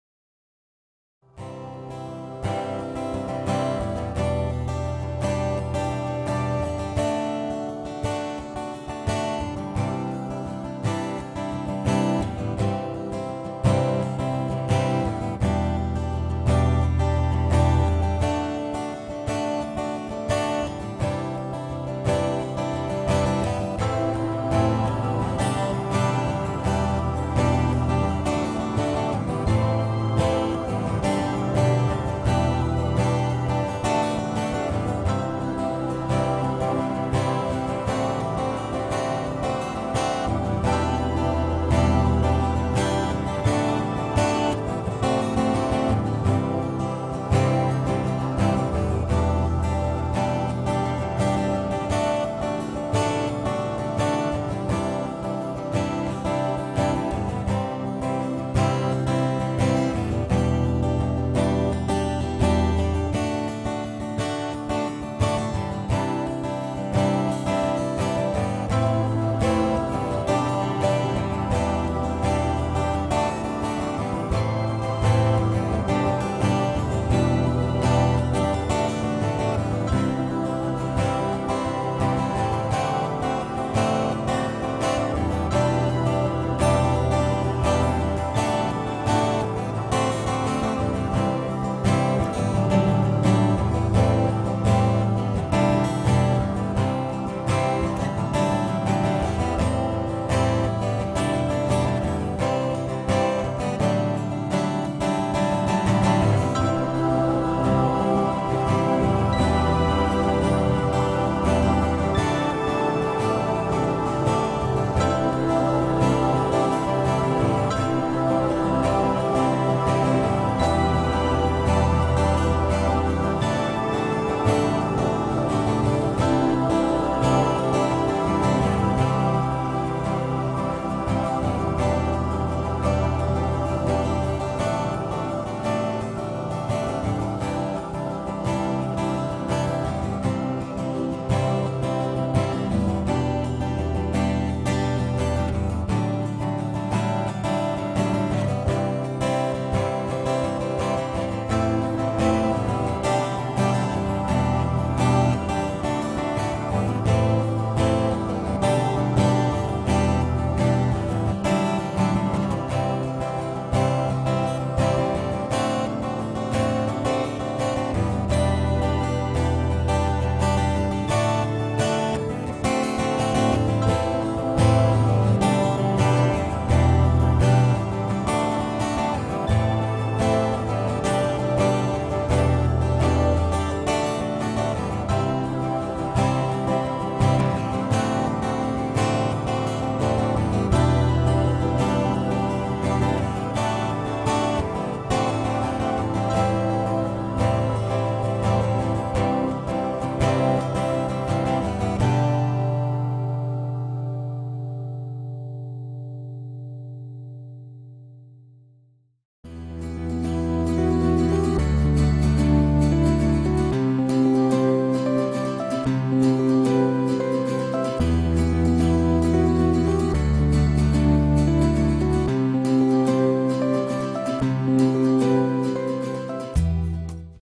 Face painting time lapse fairy by Damien Rowe
Done with snazaroo face paint ,90 mins in total ,thanks to my girlfriend for being my model ,for music